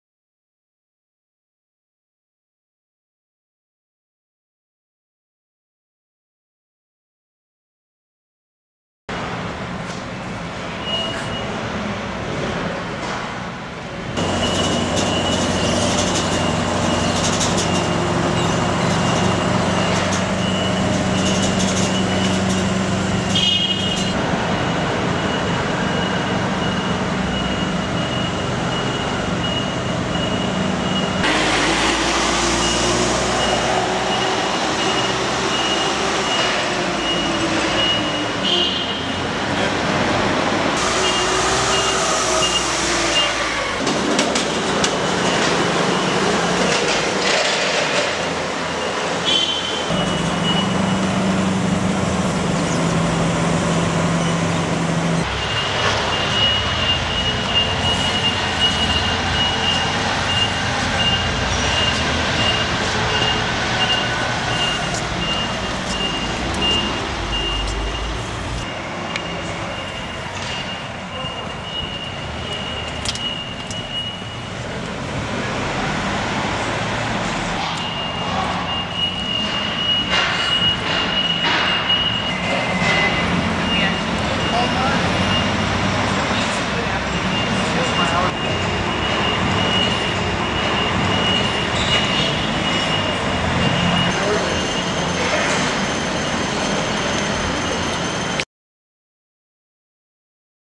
Customs agents snag 16,000 counterfeit hoverboards in Chicago
U.S. Customs and Border Protection officers recently seized more than 16,000 counterfeit hoverboards -- and perhaps prevented some unwanted fires.